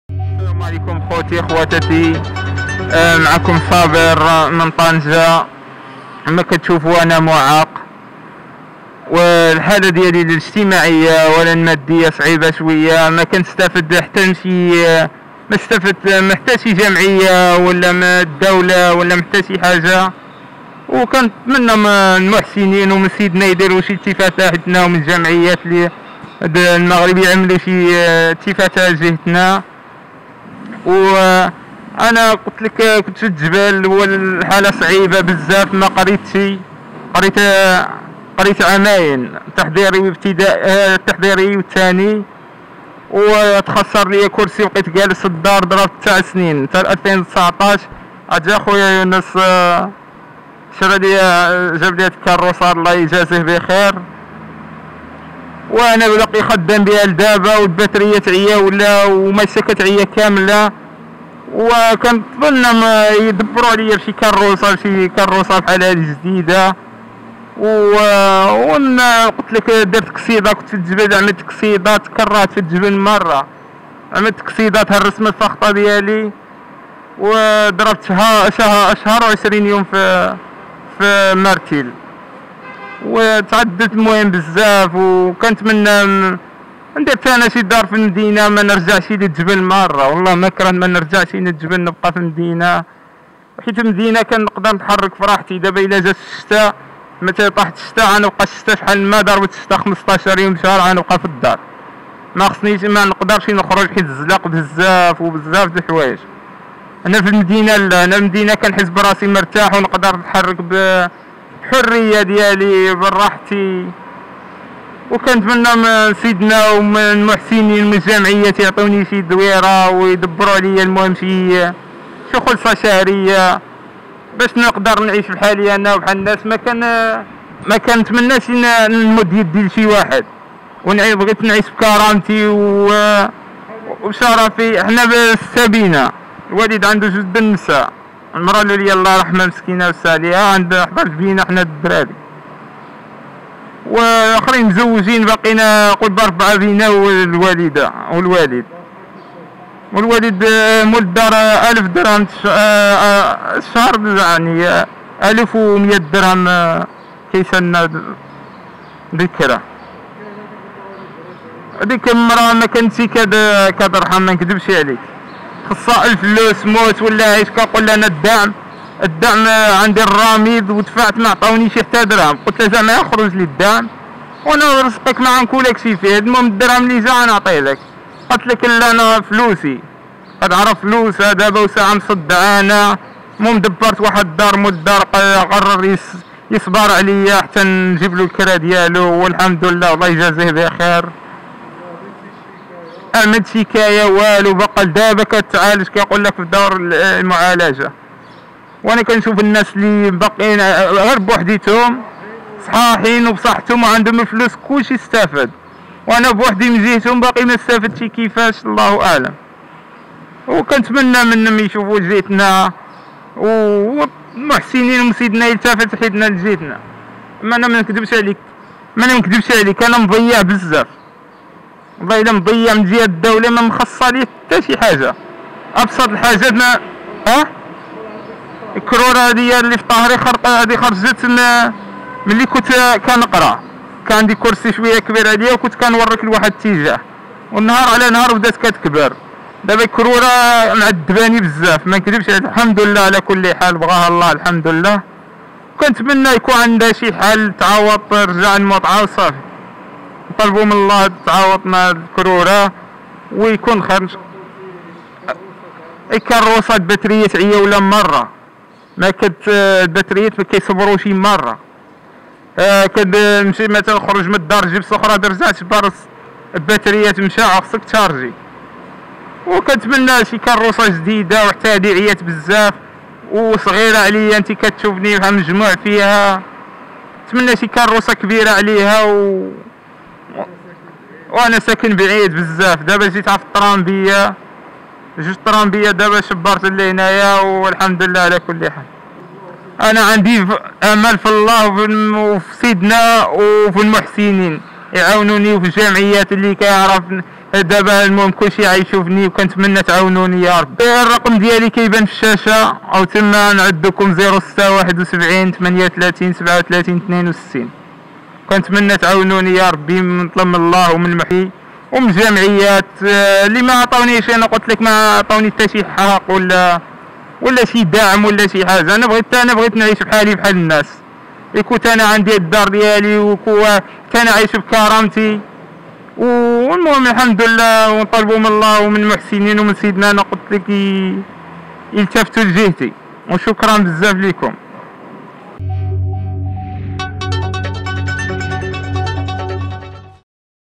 بتأثر كبير..شاب طنجاوي يعاني الإعاقة يكشف مأساته الصادمة ومعاناته مع المرض والفقر
ناشد شاب طنجاوي يعاني الإعاقة الحركية بتقديم المساعدة المادية والمعنوية له.